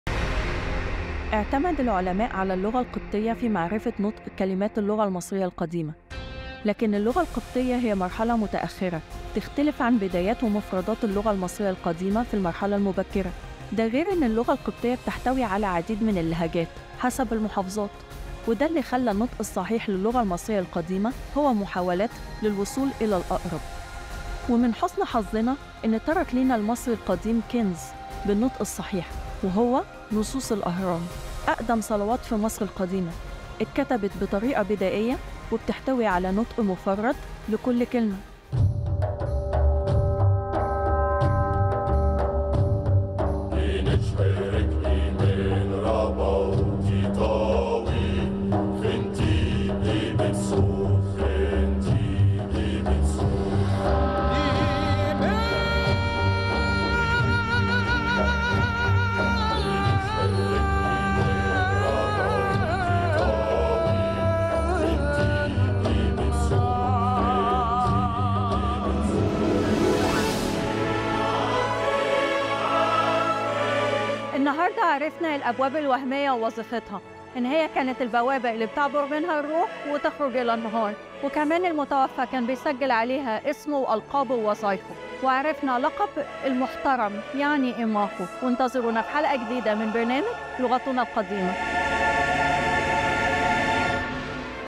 لغتنا القديمة | د. هدى عبد العزيز: اعتمد العلماء على اللغة القبطية لمعرفة نطق اللغة المصرية القديمة
لغتنا القديمة | د. هدى عبد العزيز: اعتمد العلماء على اللغة القبطية في معرفة نطق اللغة المصرية القديمة